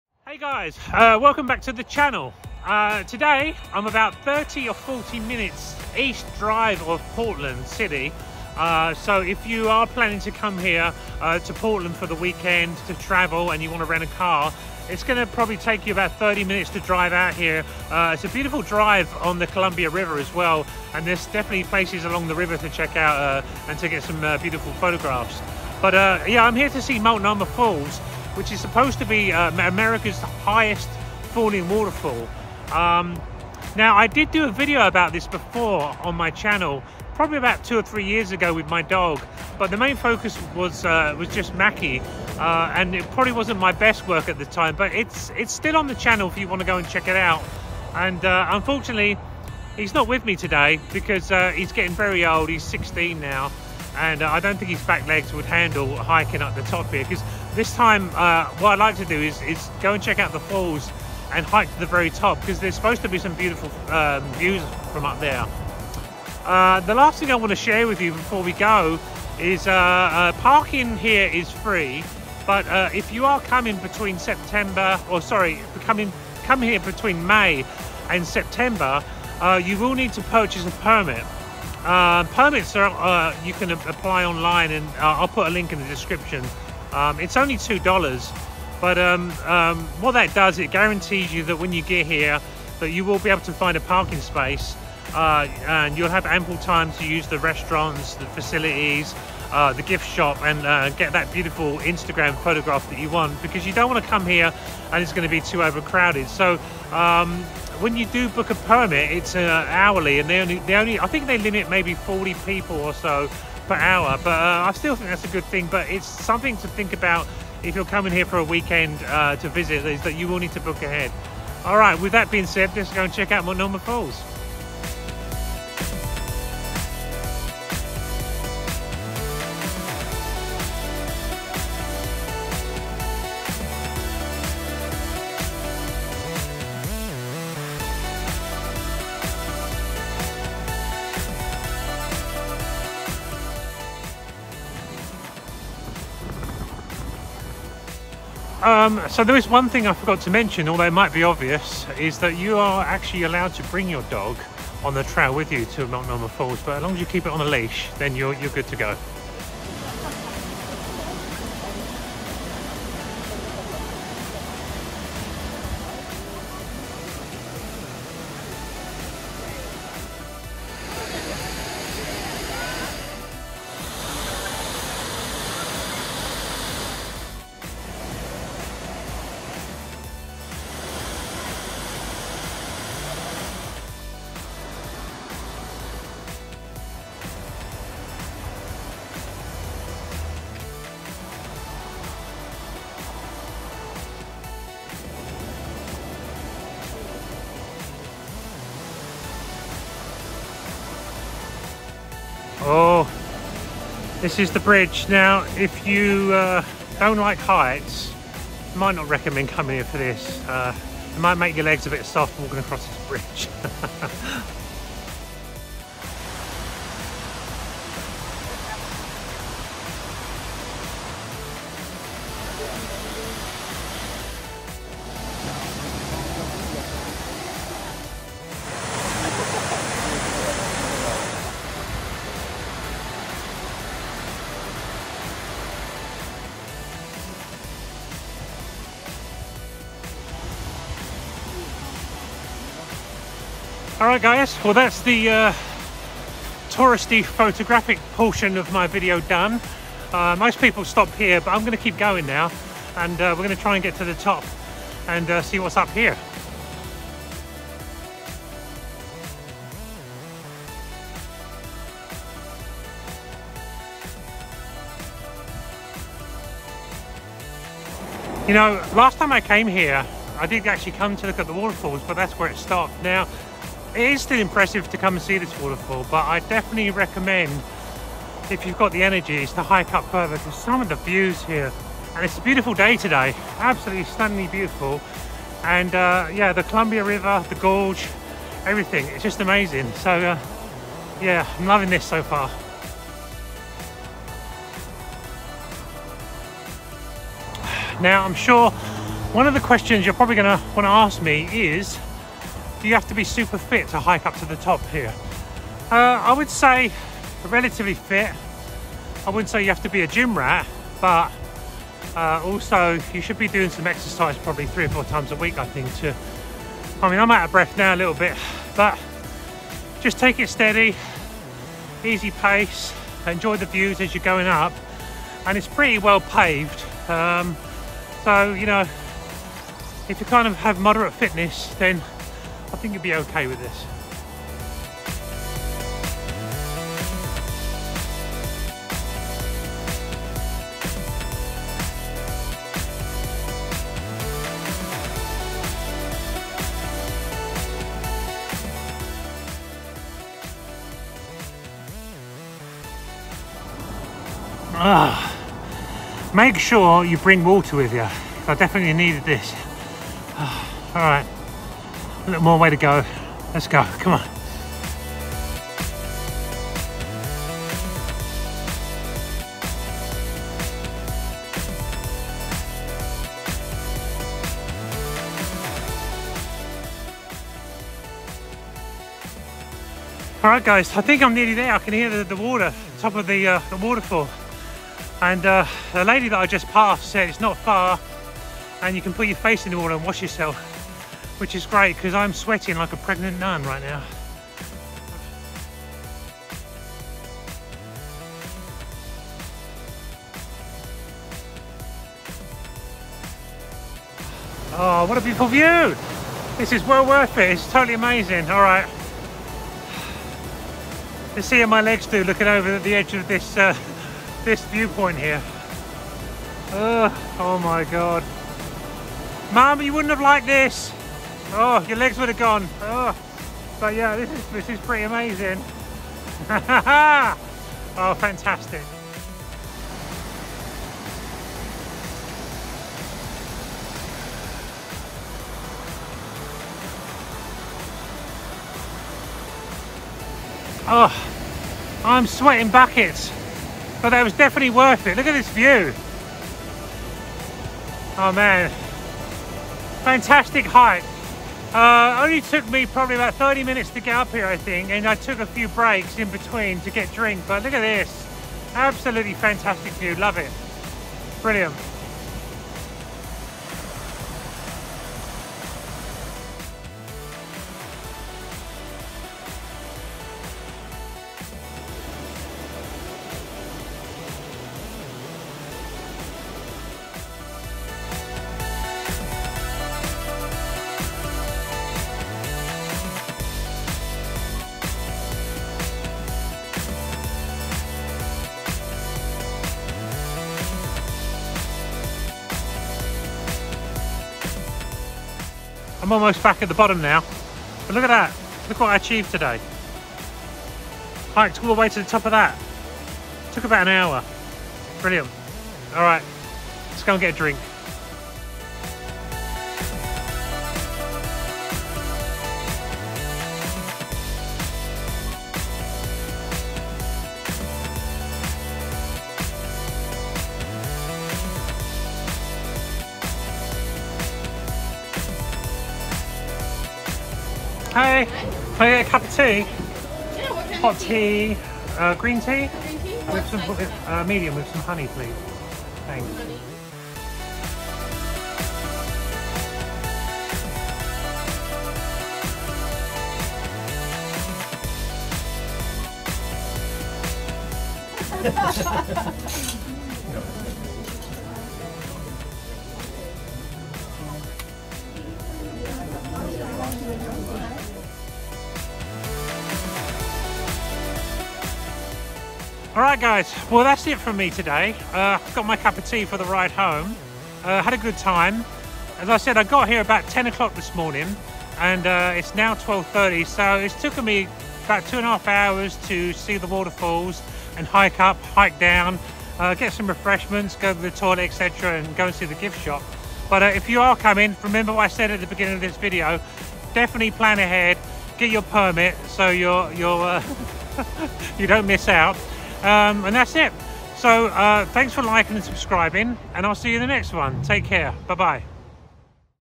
Multnomah Falls, Oregon USA
Multnomah Falls is located on Multnomah Creek in the Columbia River Gorge, east of Troutdale, between Corbett and Dodson, Oregon, United States.